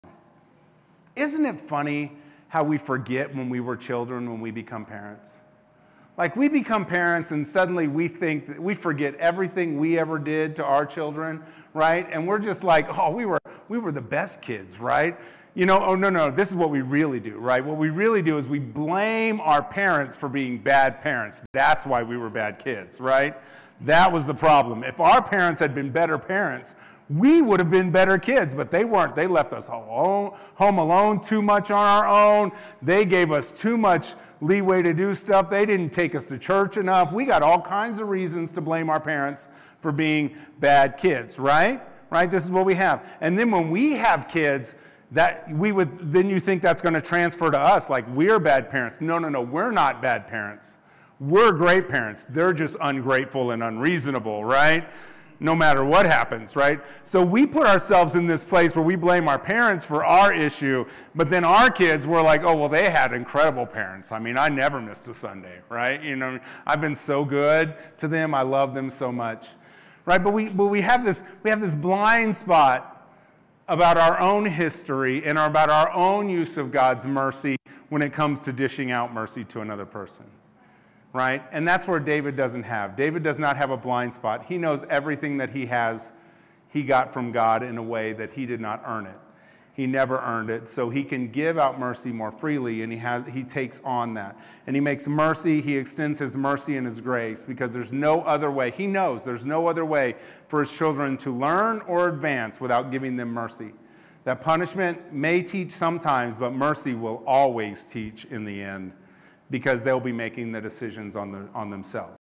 From Blaming to Building: The Importance of Parenting and Mercy
Discover how our perspective on parenting shifts when we become parents ourselves. Explore the impact of blaming our own parents and the need for extending mercy and grace to our children. Learn from David's example of understanding the significance of God's mercy in our own lives and its influence on our parenting journey.